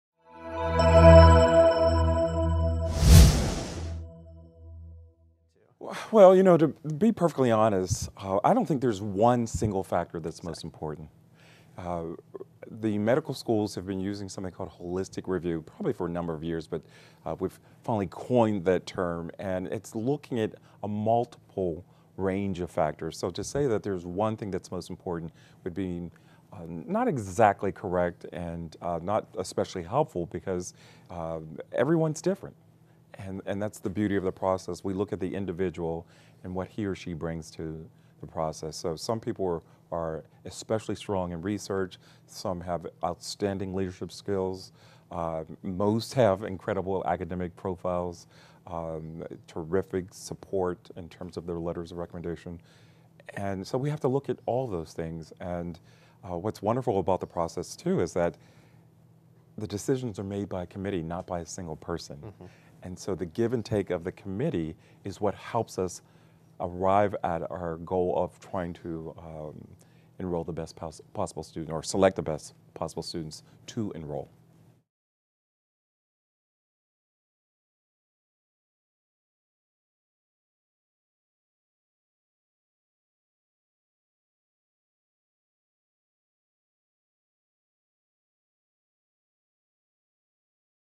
Medical School Application: What Are the Factors for Medical School Admissions? | Kaplan MCAT Prep
There's a lot to juggle when you're applying for medical school. Your pre-med classes, MCAT score, extracurriculars, etc. all play a part in how you're perceived by medical school admissions committees.  In this video you'll learn what medical schools look for when reviewing applicants. Paul T. White, J.D., Assistant Dean of Admissions at Johns Hopkins School of Medicine, discusses the holistic medical school review process.

Pre-Med Life
Medical School Interviews
Medical Careers